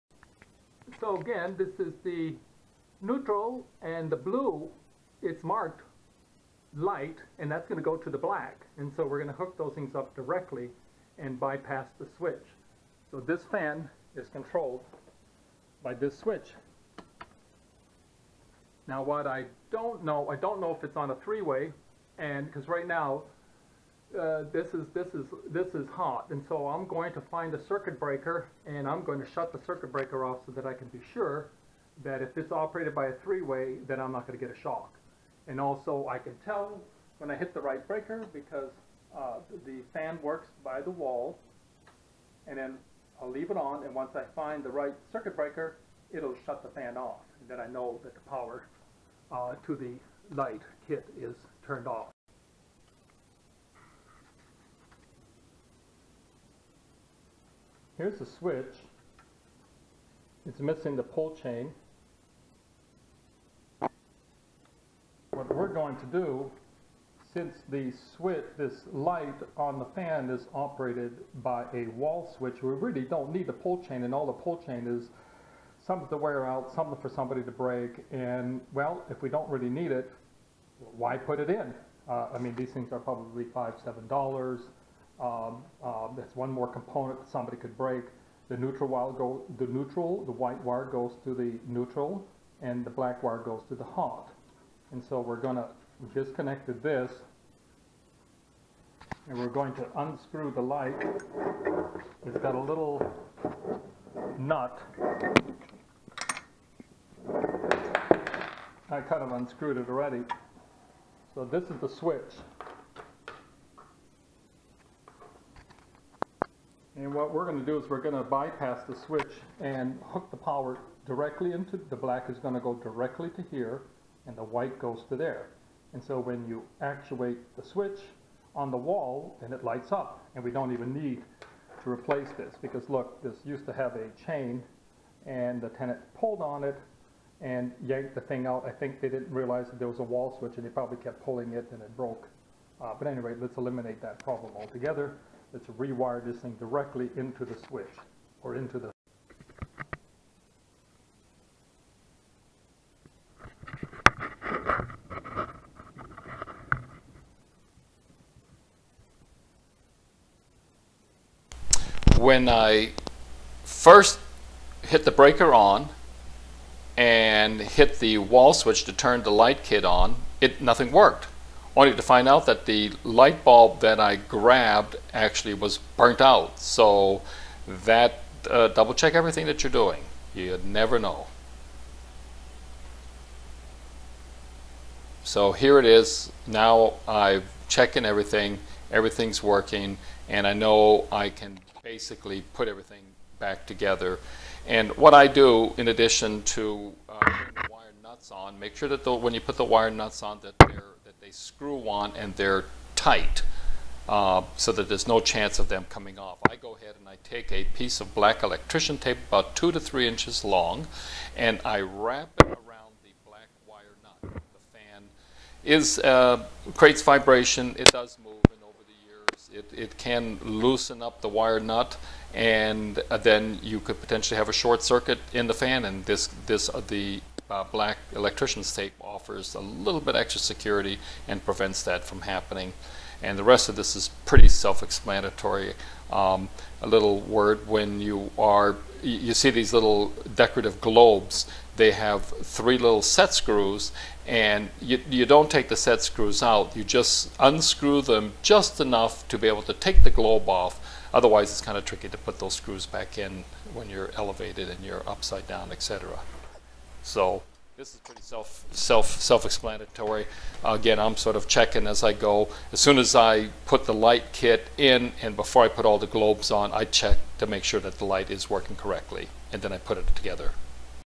fix fan light pull chain for free no need to buy any parts
This hack only works when the fan light is controlled by a wall switch. The wall switch should be a 2 gang where 1 switch controls the fan the other switch controls the light. So if your fan does not have a wall switch you can't use this method. Otherwise what happens is at night when you shut the light off the fan stops working. So the fan has to have either have a constant hot wire or be controlled  by a wall switch.  To keep it simple if your fan/light has 2 wall switches ,1 for the light, 1 for the light then this hack works great